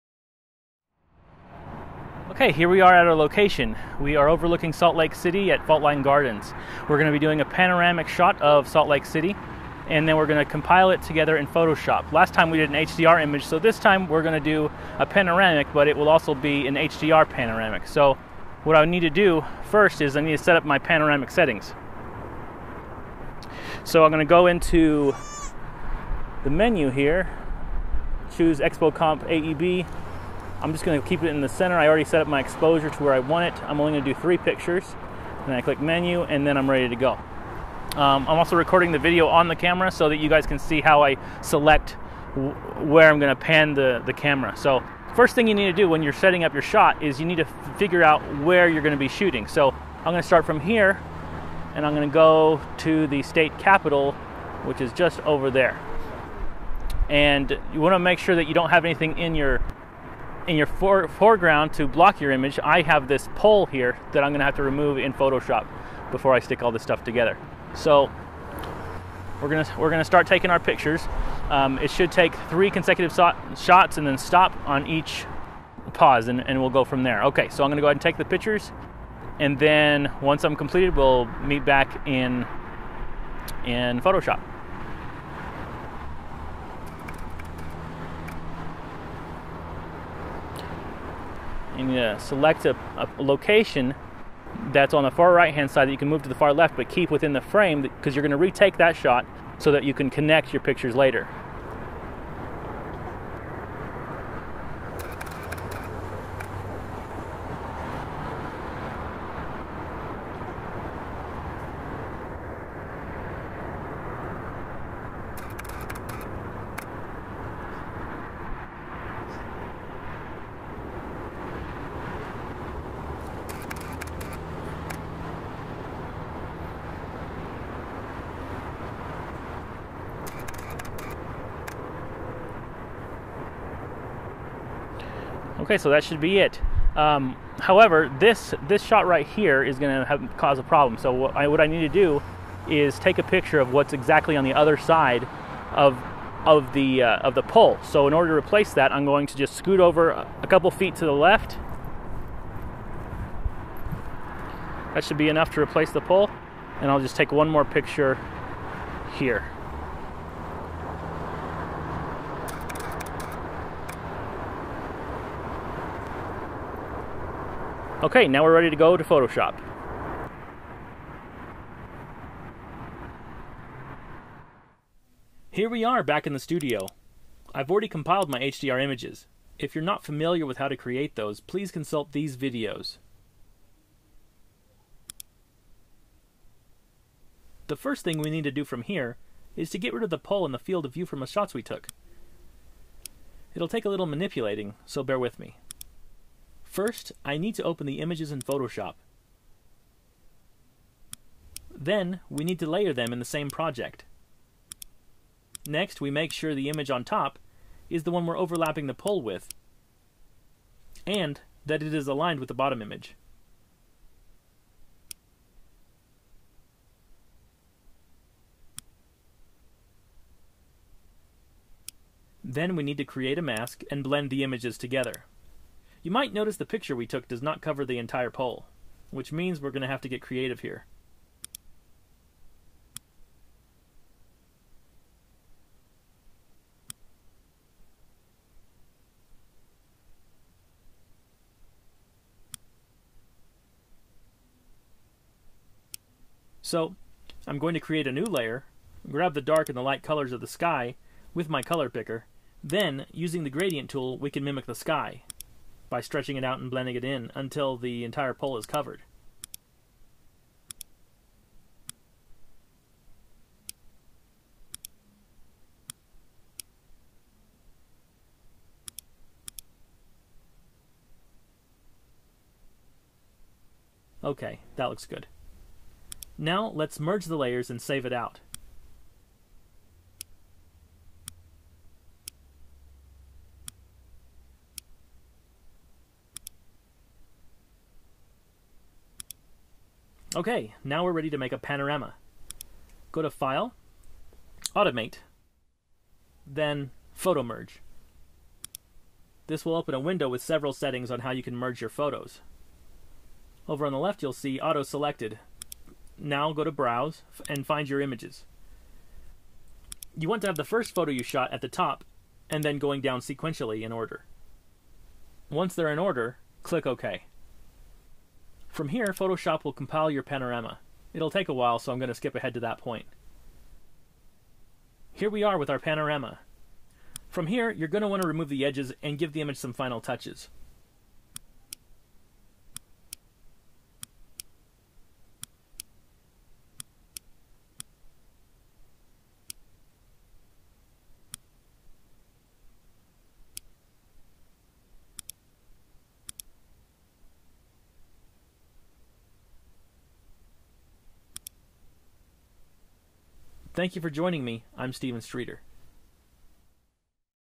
How To Panorama HDR Style w/Canon 5D and GoPro
I walk you through capturing images to compile into a panorama. We then work in photoshop to remove an obstruction in the foreground, and create a panorama.

You can see this picture along with others at my photo stream on Flickr: